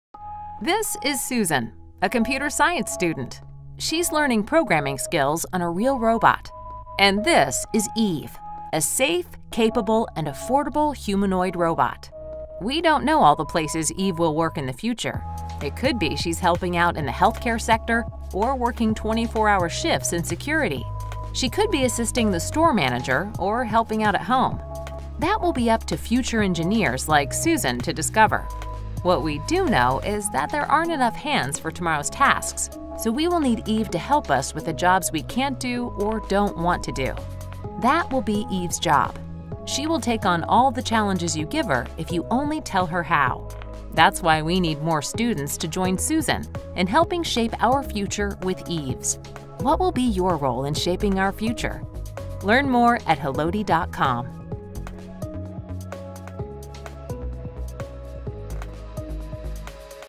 🎬 Робот-гуманоид на колесах. | НАУКА 4 0
🎬 Норвежский самобалансирующийся гуманоид на колесах EVE r3, способен выполнять самые разные задачи, чтобы помогать ученым и пожилым людям. | НАУКА 4.0